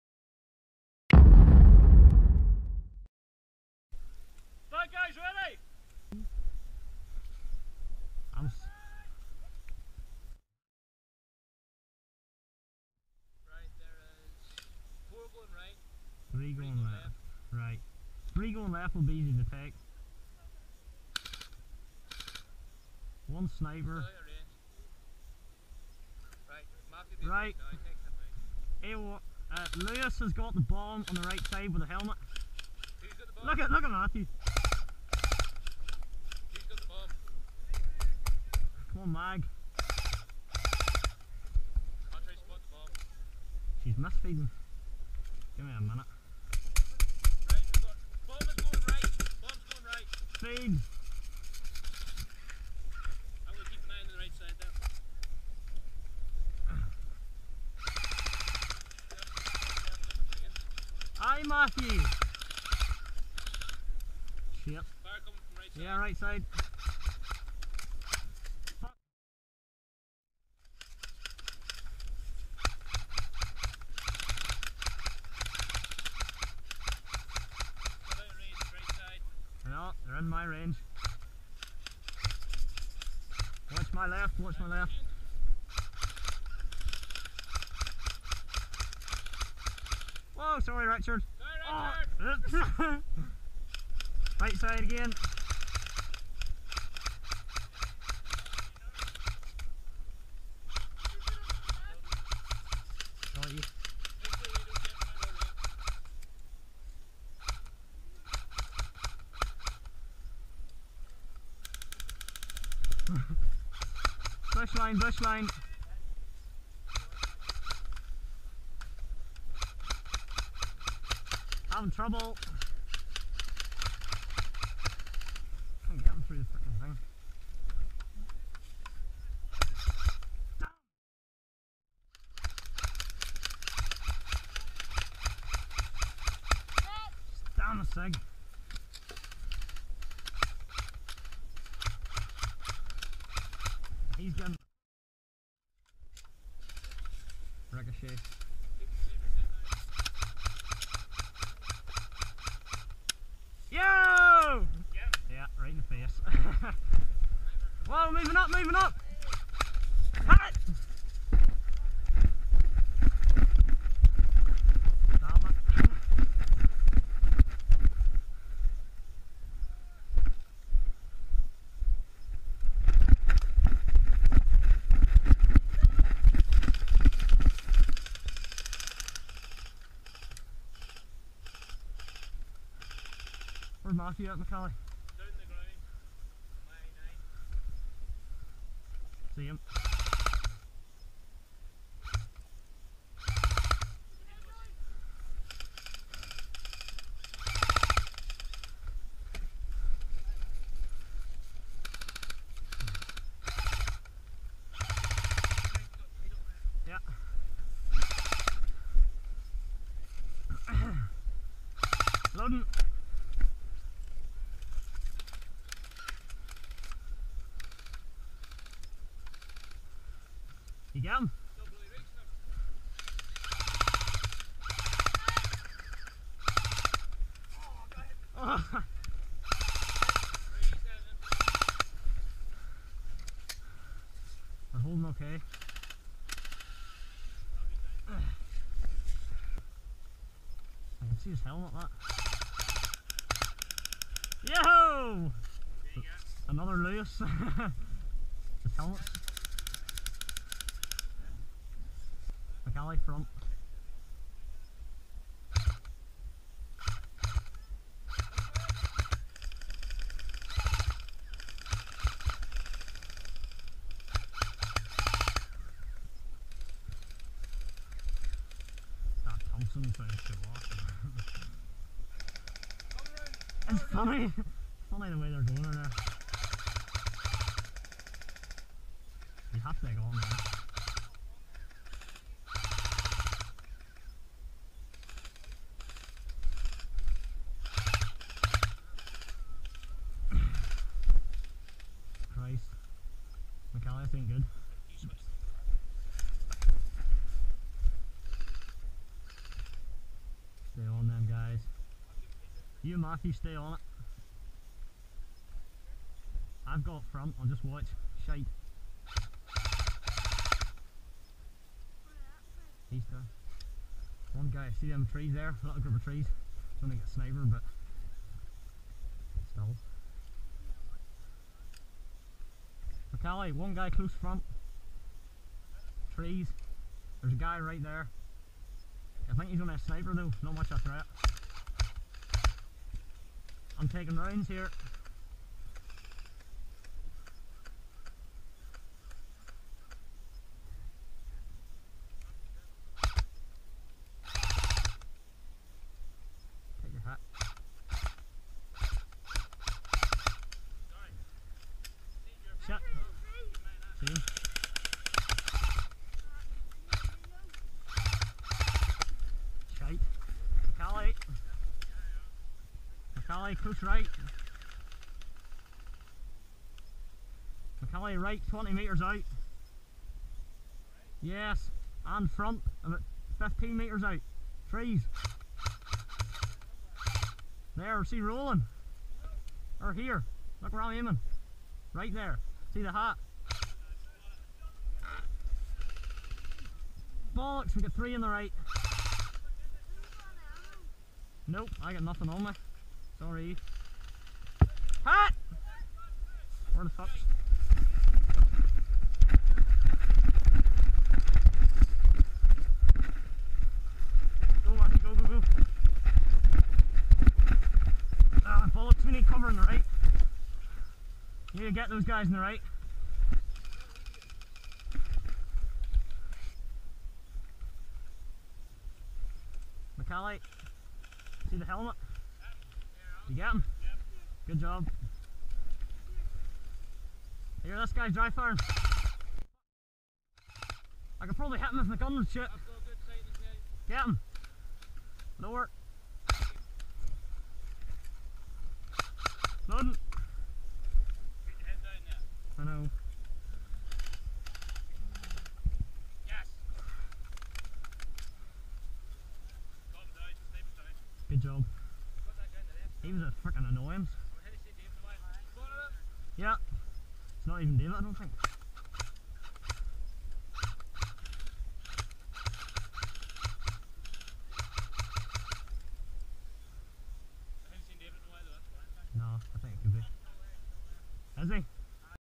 CFZ Airsoft | Defend the FOB Phase 3
Operation Tiger Strike Pre-Games. @ Conflict Zone Airsoft Ballybogey
Note the site is not fully built and finished. thus why the FOB has the left side of it fallen down ready to be propped up.